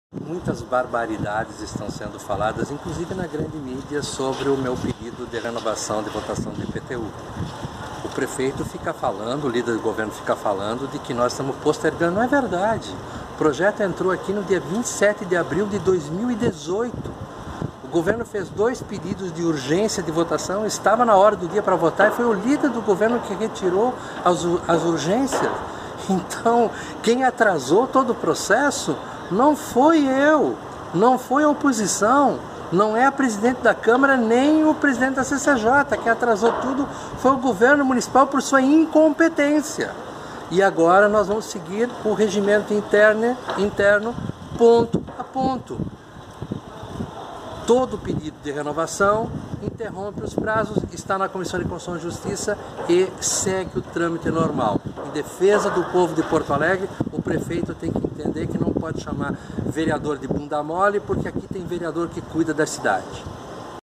IPTU: a prefeitura tropeça nas próprias pernas
Ano passado a base do governo retirou duas vezes o projeto do IPTU da pauta aqui na Câmara. Agora que estamos pedindo a renovação de votação, com base no regimento interno da Câmara, nos acusam de atrasos.